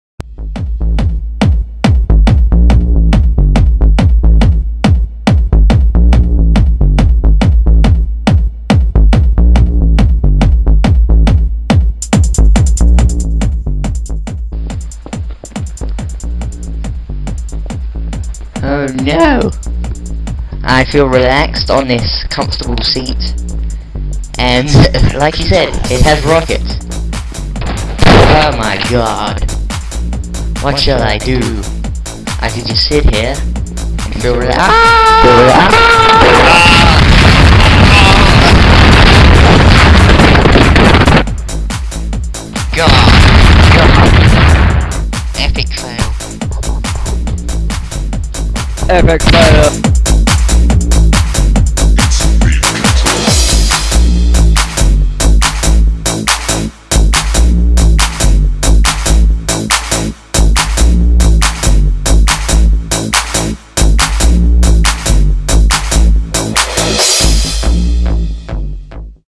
The Seat
Me and Sam a.k.a {TG} Player now decided to record at the same time at the airport. This is the infamous seat scenario.